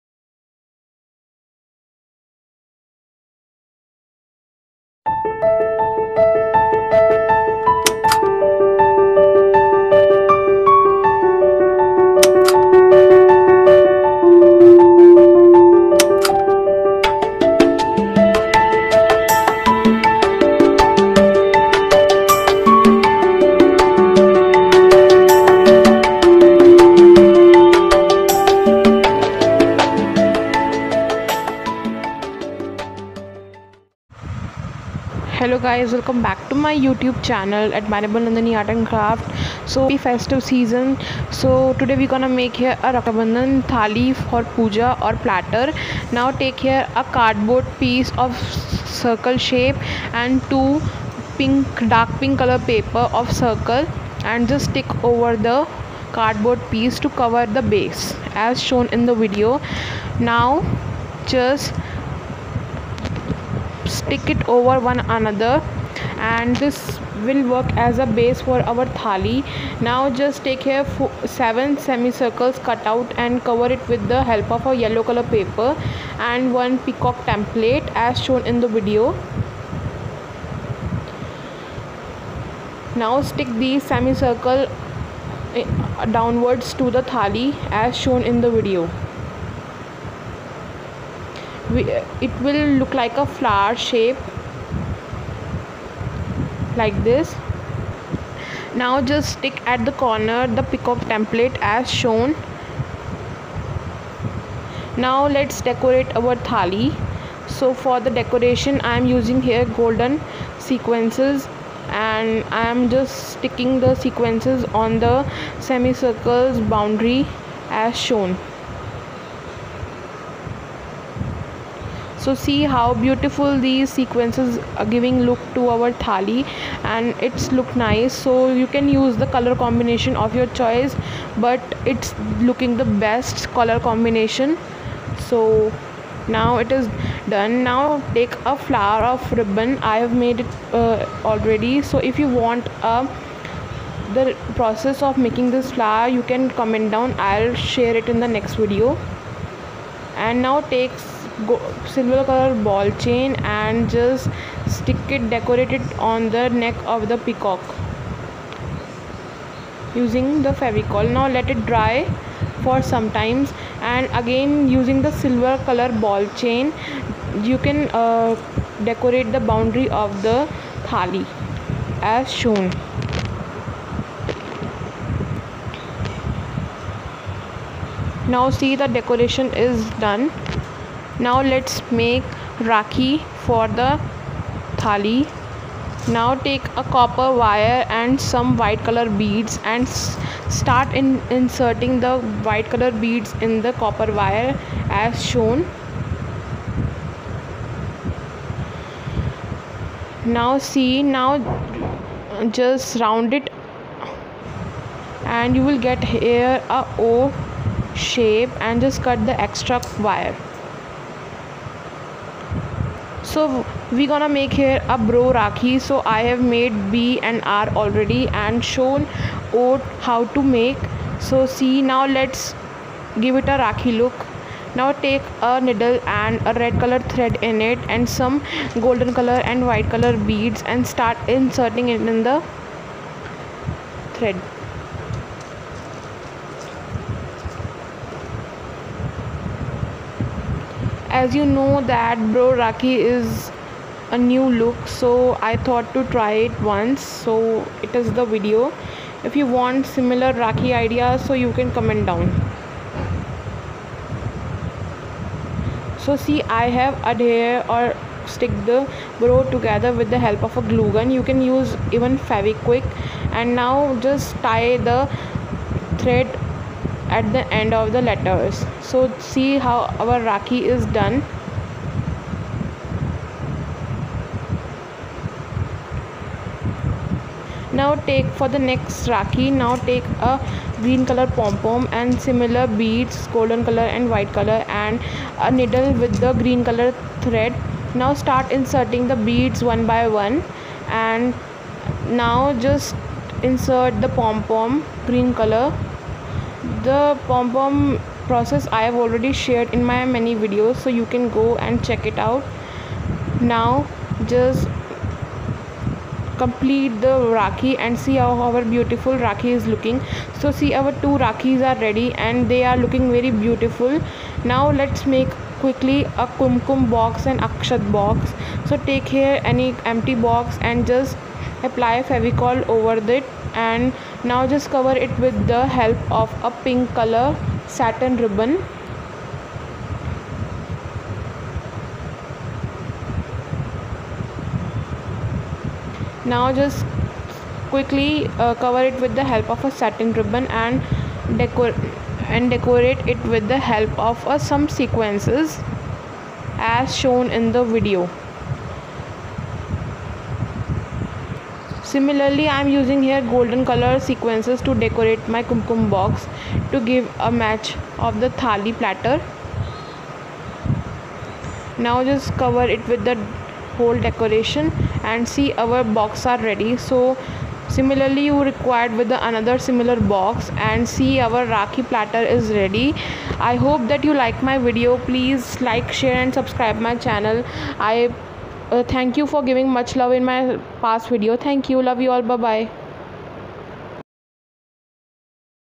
Raksha Bandhan platter ll pooja thali making ll DIY rakhi ll kumkum roli box ll ❤🌸
*Happy festive season!!*

In today's u gonna watch Raksha Bandhan thali platter making with the making of Diy rakhis at home with limited materials ⭐🌈

May this festival bring lots of happiness in ur life.. 😇🌝
Hope that u liked my vedio ☺☺

@Admirable Nandini art and craft 🧡 🌸